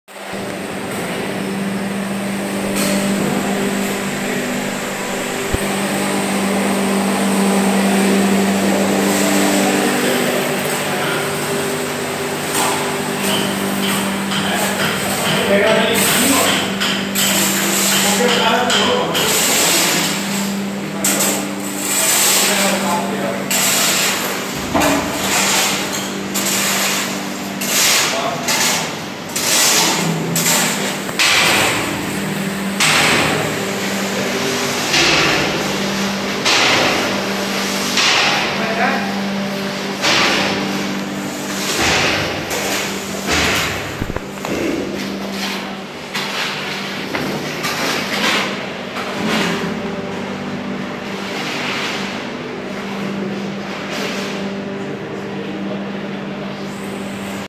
Vídeo mostra presos reformando as celas da Penitenciária Central do Estado em Cuiabá
A reportagem do Lapada Lapada teve acesso a um vídeo que mostra presos trabalhando na reforma de celas da Penitenciária Central do Estado em Cuiabá.

A unidade conta com aproximadamente 2.400 presos cumprindo pena em regime fechado, mesmo com capacidade para apenas 900.

Os presos estão tapando buracos nas paredes, retirando a rede de energia elétrica e pintando as celas. A cada tres dia trabalhados na reforma dos raios da PCE um dia é descontado da pena do detento.

ACESSE NOSSO PORTAL E FIQUE BEM INFORMADO